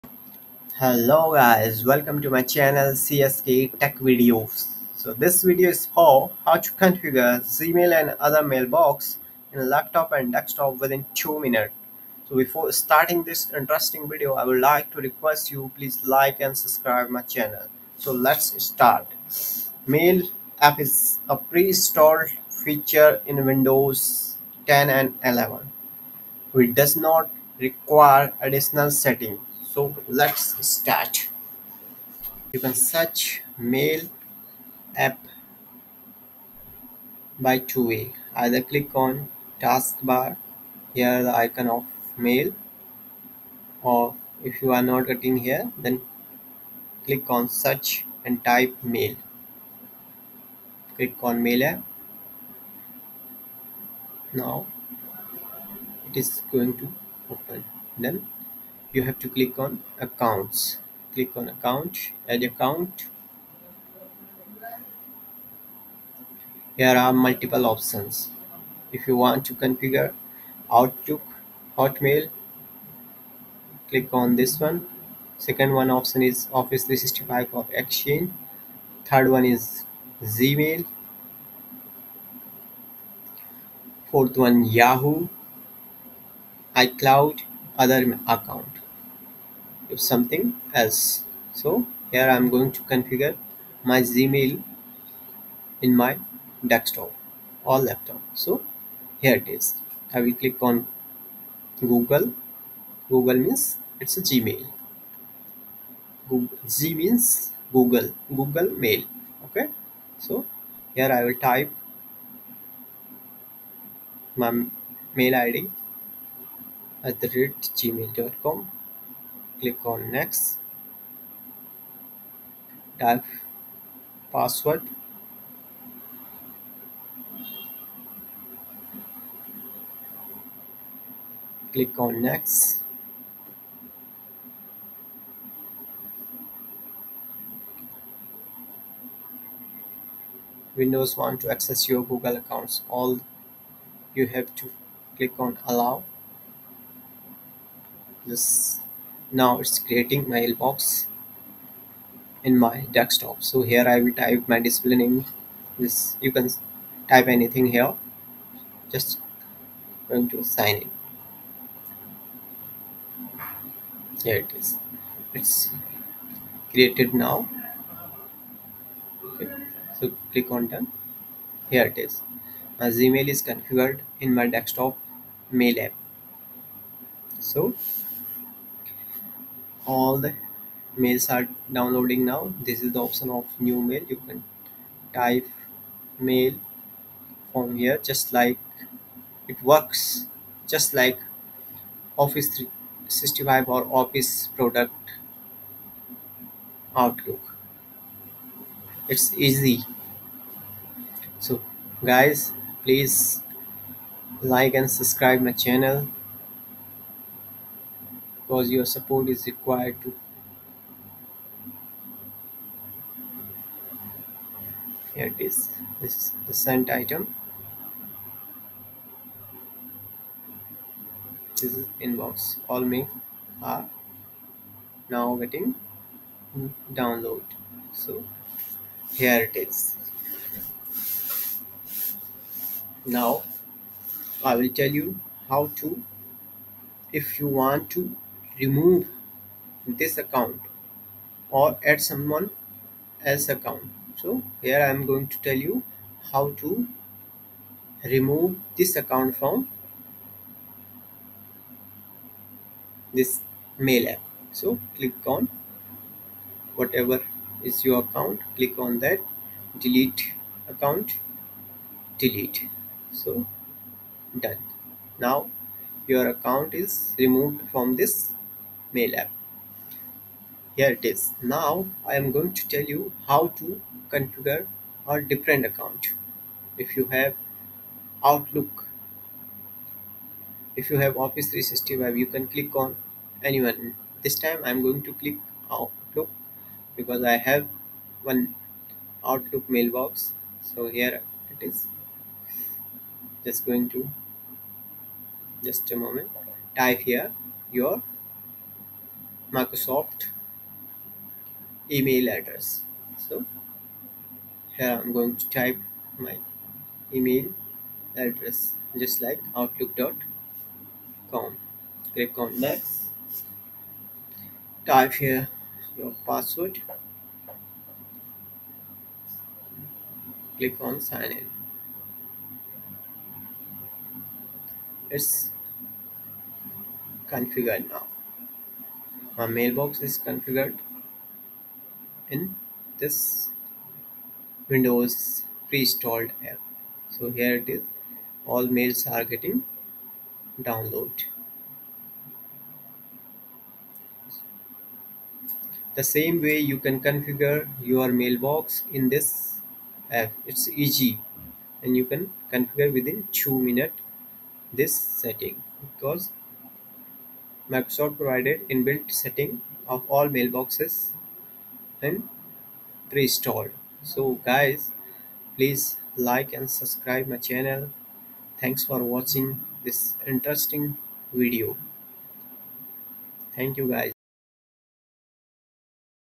How to configure Gmail Mail | How to configure outlook in Windows 11| Windows 10 | Windows Mail app
CSK tech videos YouTube channel for technical assistant|

If you have Windows 10 or a later version and you sign in with a Microsoft account that has an Outlook.com, Live, Hotmail, or MSN address, that account is added to the Mail and Calendar apps. You can add that email account and many other email accounts to the Mail and Calendar apps to start sending and receiving email and creating and managing events.